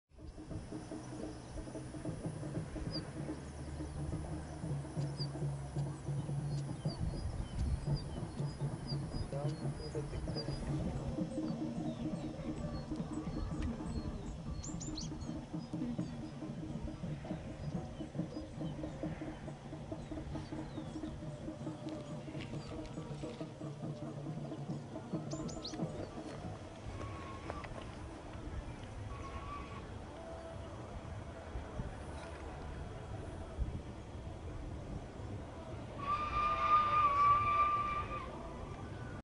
Indian Coral tree flowering in the Ganga valley
Erythrina indica or the Indian Coral tree flowering gloriously along the banks of the then-peaceful Ganga or Ganges river, as seen below Uttarkashi town, Uttarakhand, India.